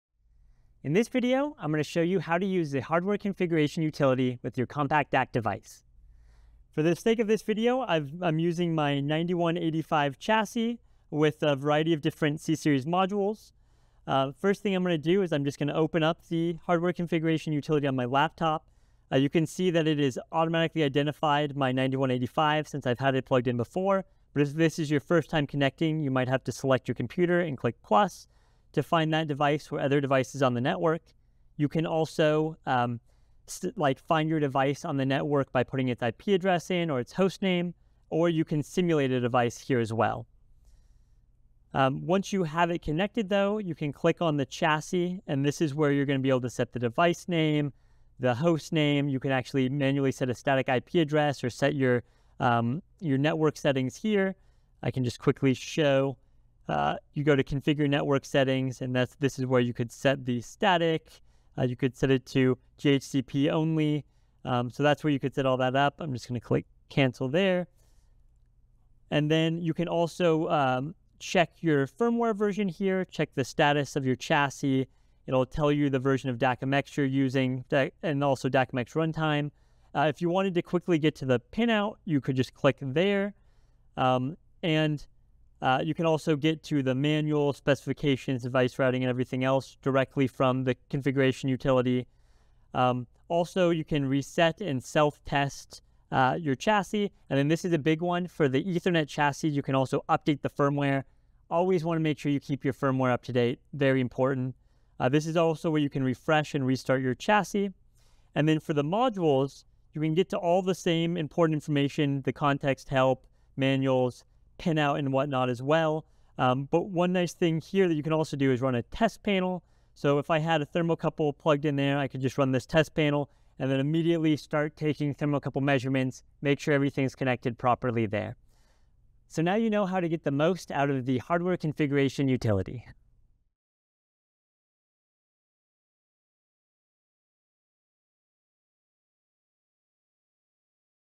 Setting Up a NI CompactDAQ with the Hardware Configuration Utility
See how to detect and configure your CompactDAQ hardware in NI’s new hardware configuration utility, which replaces much of NI MAX functionality. Learn about test panels, pinouts, firmware updates, and where to find manuals or specifications—all in one place.